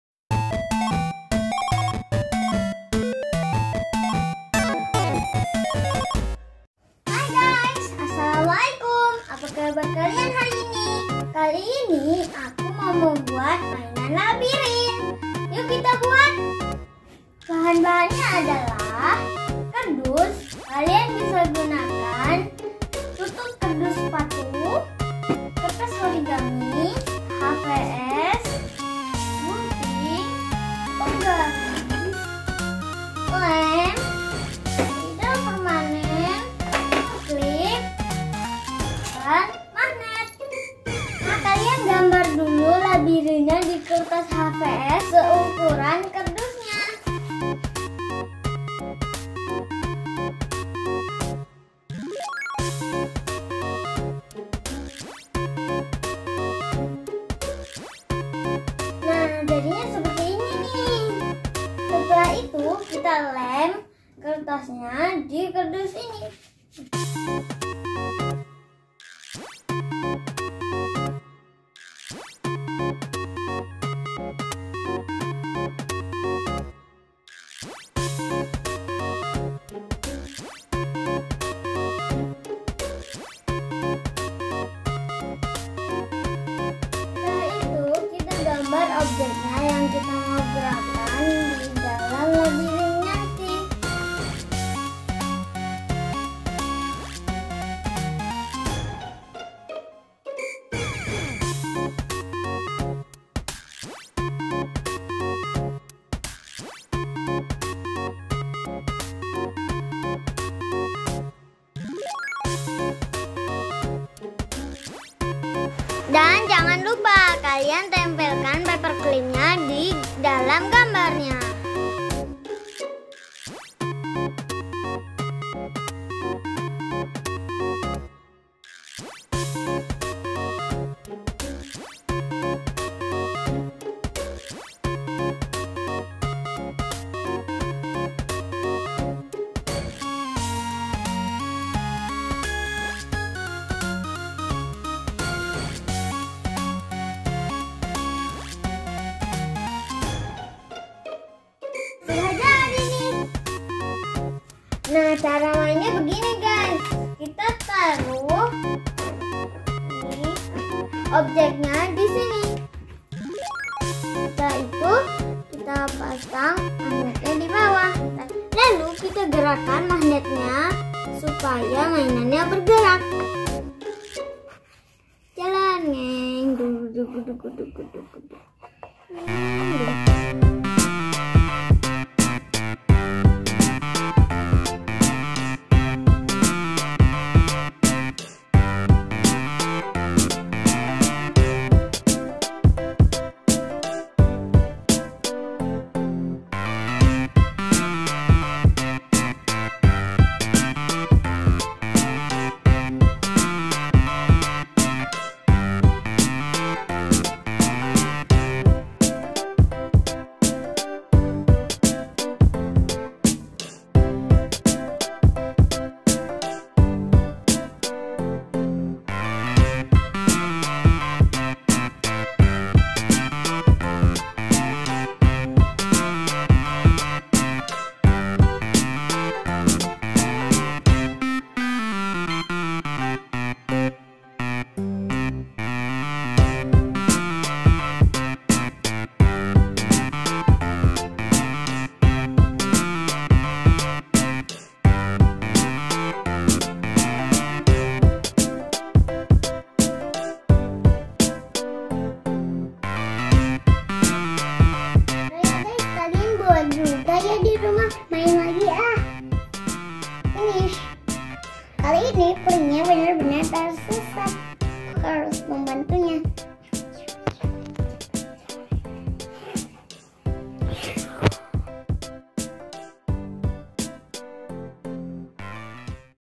DIY maze
Kali ini Zaidan mau buat mainan labirin bersama bunda.
Menggerakkan karakter Among us, roket dan peri.
Seru...

Bahannya juga mudah didapat.
Kalian coba buat juga ya...